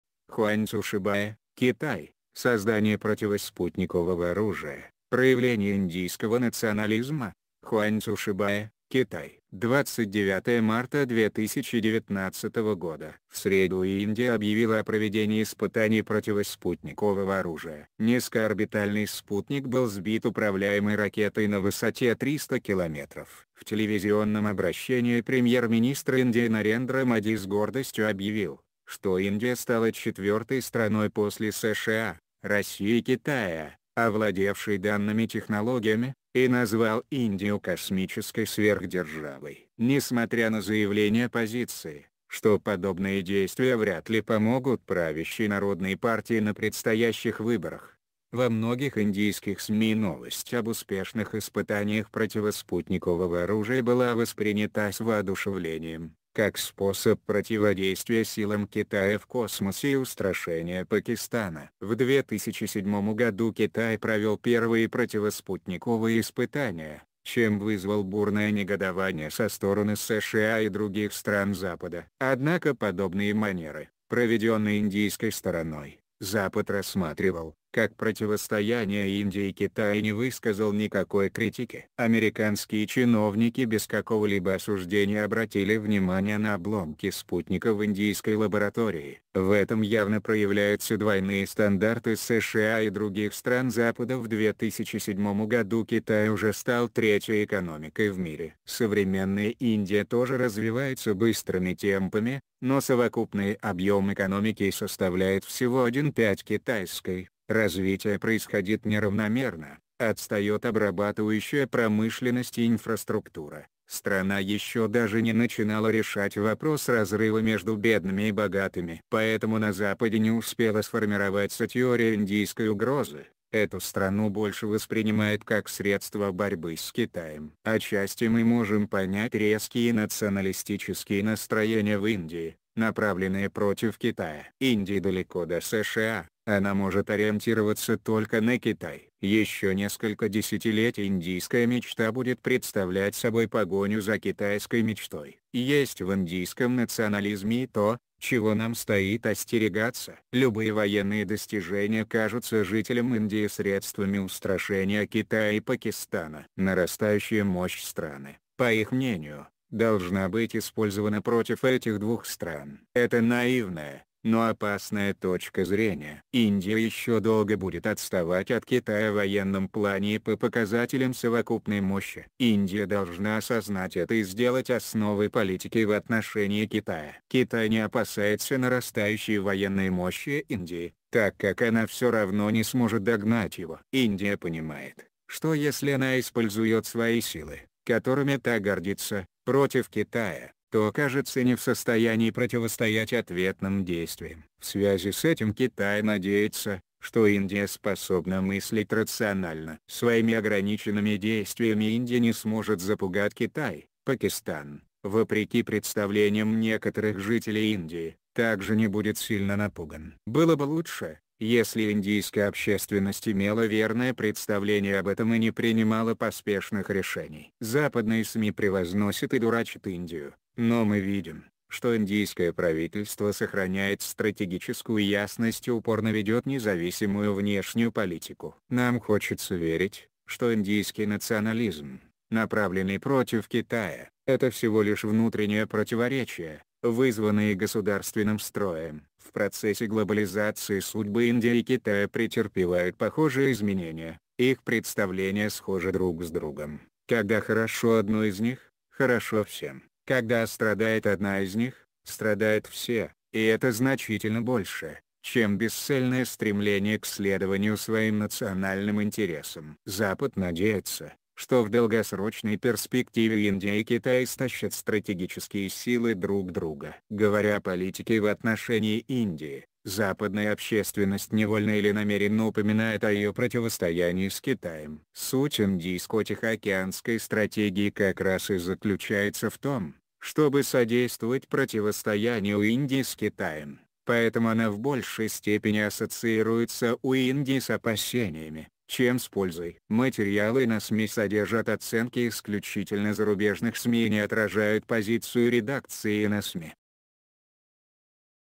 Хуаньцю шибао (Китай): создание противоспутникового оружия — проявление индийского национализма?.
Хуаньцю шибао, Китай.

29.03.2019

В среду Индия объявила о проведении испытаний противоспутникового оружия. Низкоорбитальный спутник был сбит управляемой ракетой на высоте 300 километров. В телевизионном обращении премьер-министр Индии Нарендра Моди с гордостью объявил, что Индия стала четвертой страной после США, России и Китая, овладевшей данными технологиями, и назвал Индию космической сверхдержавой.  Несмотря на заявления оппозиции, что подобные действия вряд ли помогут правящей народной партии на предстоящих выборах, во многих индийских СМИ новость об успешных испытаниях противоспутникового оружия была воспринята с воодушевлением, как способ противодействия силам Китая в космосе и устрашения Пакистана.  В 2007 году Китай провел первые противоспутниковые испытания, чем вызвал бурное негодование со стороны США и других стран Запада. Однако подобные манеры, проведенные индийской стороной, Запад рассматривал, как противостояние Индии и Китая и не высказал никакой критики. Американские чиновники без какого-либо осуждения обратили внимание на обломки спутника в индийской лаборатории. В этом явно проявляются двойные стандарты США и других стран Запада  В 2007 году Китай уже стал третьей экономикой в мире. Современная Индия тоже развивается быстрыми темпами, но совокупный объем экономики составляет всего 1/5 китайской, развитие происходит неравномерно, отстает обрабатывающая промышленность и инфраструктура, страна еще даже не начинала решать вопрос разрыва между бедными и богатыми. Поэтому на Западе не успела сф...